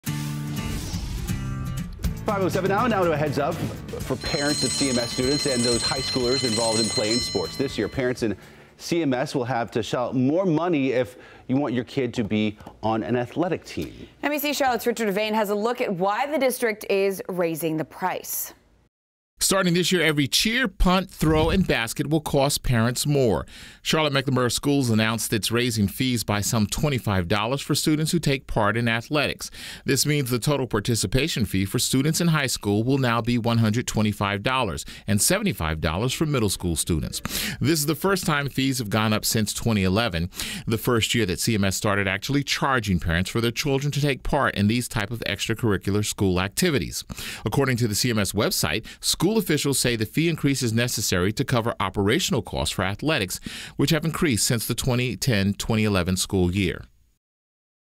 CMS athletic fee increasing
Parents of CMS students will be required to pay more money for their kids to play sports this school year.